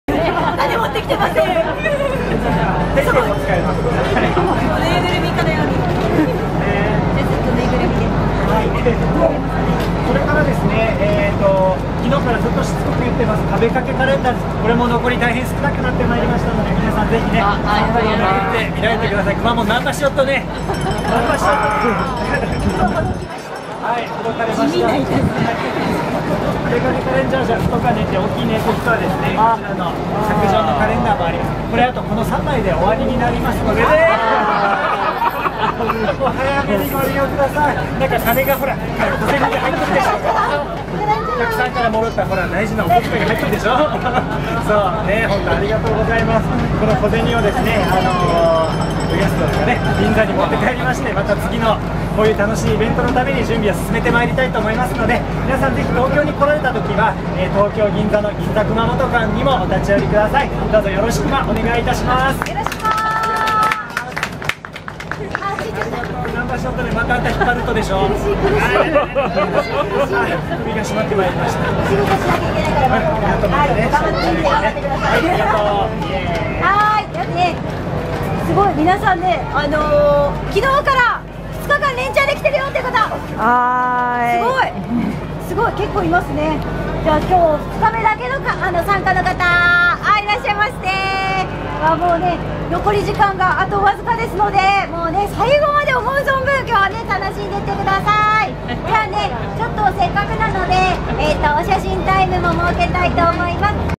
ゆるキャラグランプリ2019inしあわせ信州NAGANO。くまモン
ゆるキャラグランプリ2019inしあわせ信州NAGANO。くまモンブースの様子。